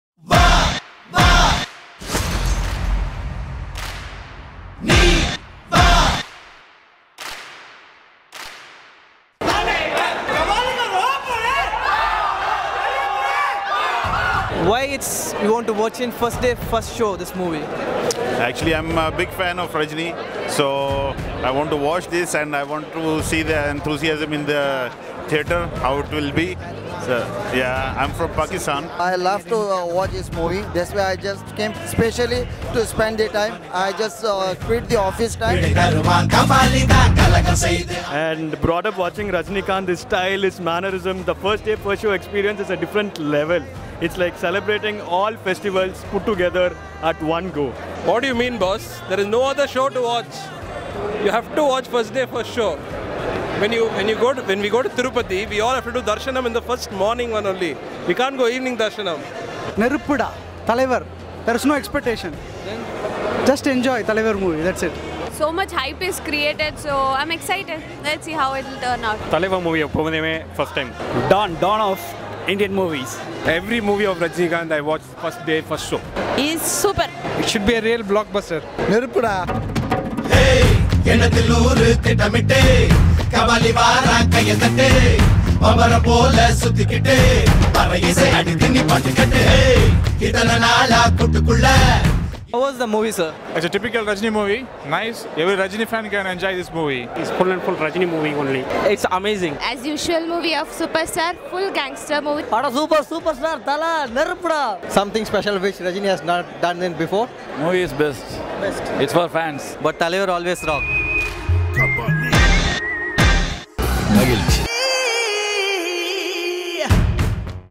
KABALI - "Dubai" FDFS Reaction & Review | Lebara Play | Fan reactions
Also, Enjoy our Kabali Offer : FREE 30 days trial - TAMIL TV & LATEST MOVIES.
to avail this offer (excluding India)

‪Kabali DUBAI FDFS Fan Reactions & Reviews ‪
#‎PlayDaKabaliDa‬ ‪#‎LebaraPlay‬